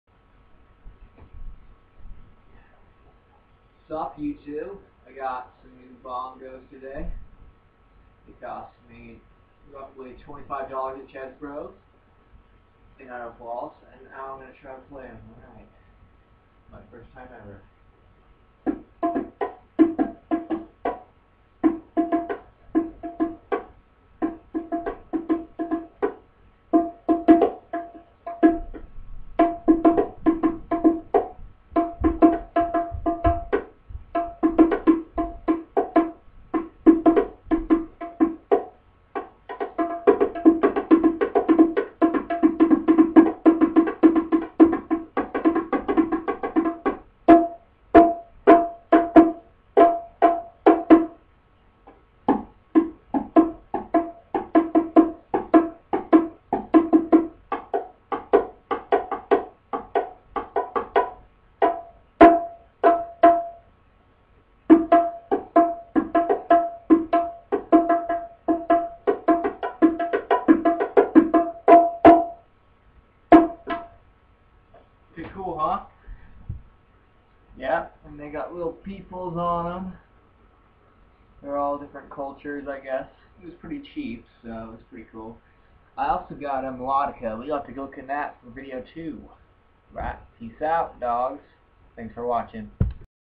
New Bongo Drums
Just bought a brand new set of bongos today and I thought I'd try them out. They may not be fancy, but they sound great for only twenty five bucks.